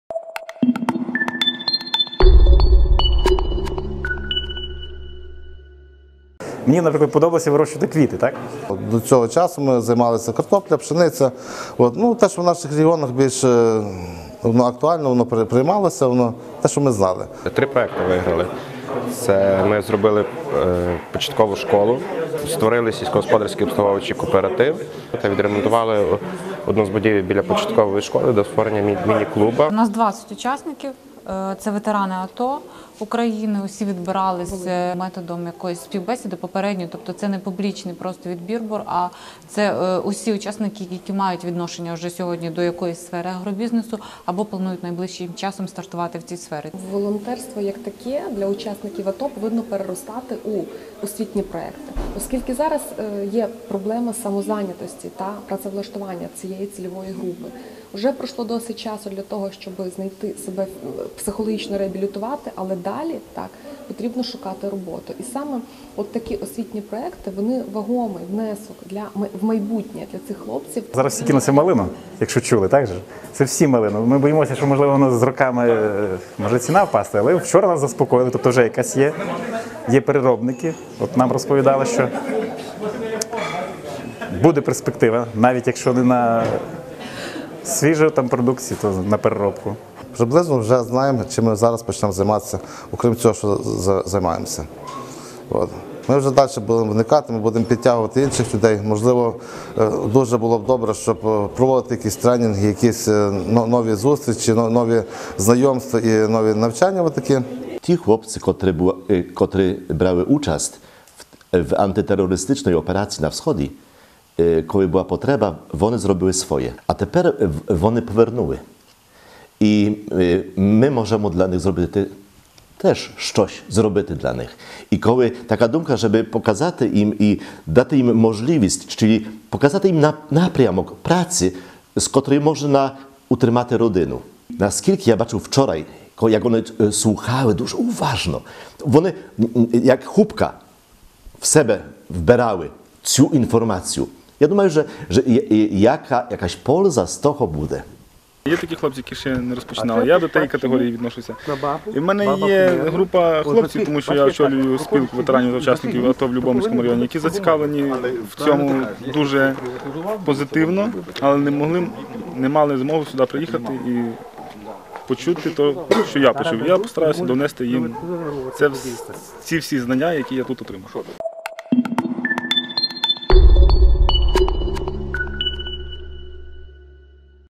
На Волині провели агрофорум для учасників АТО
Перший Міжнародний Агрофорум «Аграрії – потенціал України. Вектор на експорт» відбувся на базі інтеграційного центру «Замлиння» на Любомльщині. Форум для волинських атовців тривав з 26 по 30 січня і зібрав більше двох десятків учасників з усієї області.